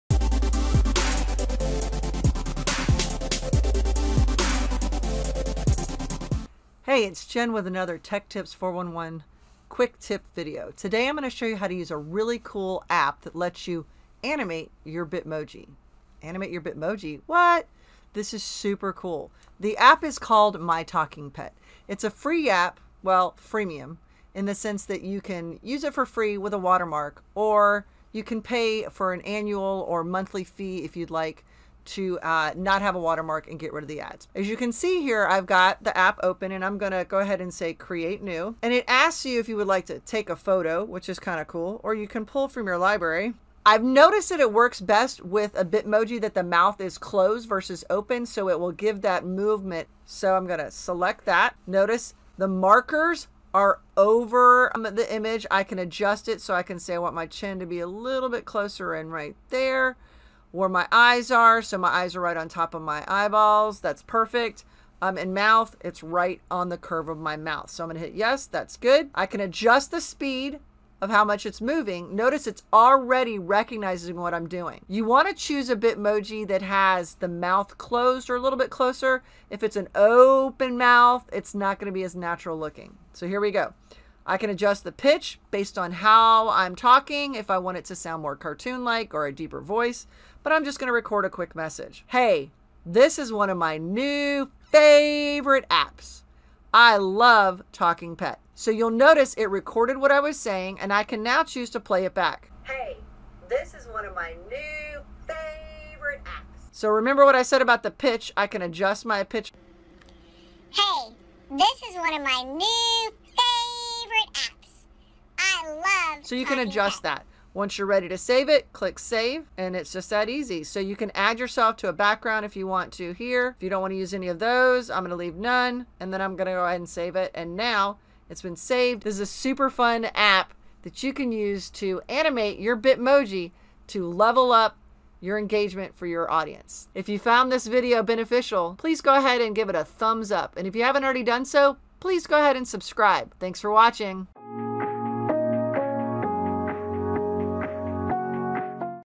How to Create a Talking Bitmoji | Quick Tip
How to use the mobile app My Talking Pet app to animate your Bitmoji.